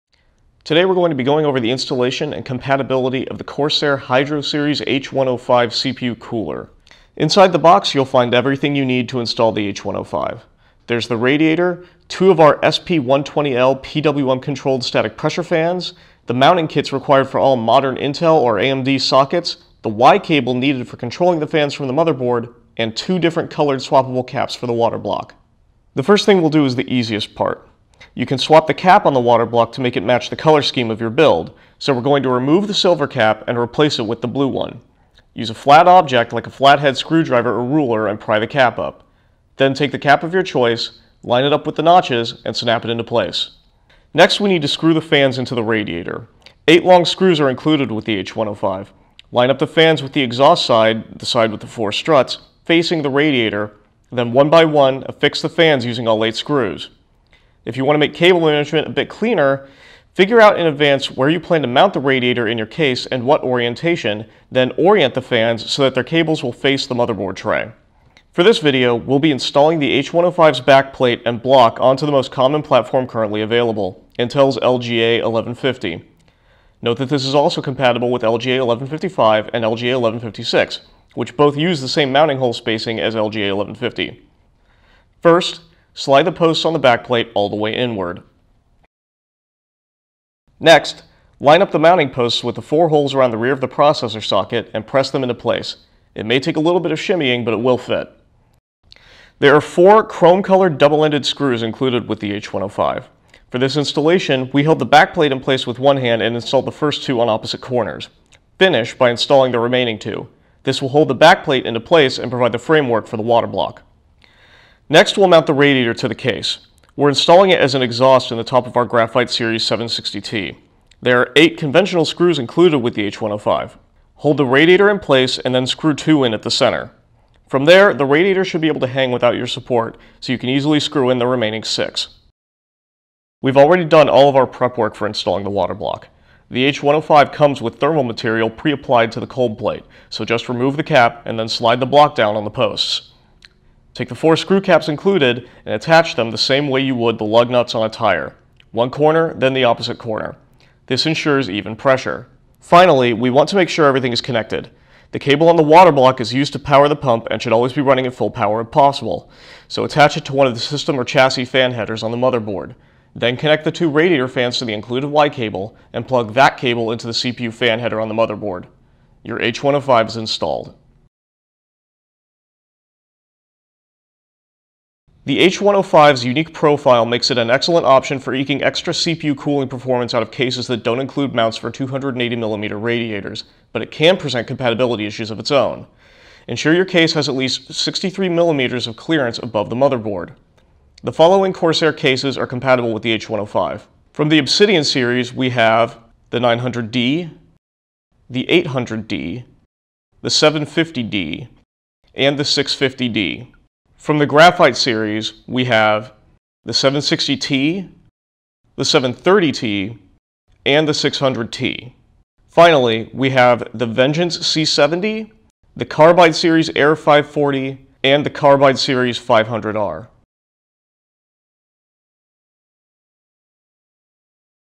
How To: Installation and Compatibility of the Corsair Hydro Series H105 Liquid CPU Cooler
Dustin Sklavos gives you step-by-step instructions on how to install the Corsair Hydro Series H105 liquid CPU as well as a brief list of Corsair PC cases readily compatible with it.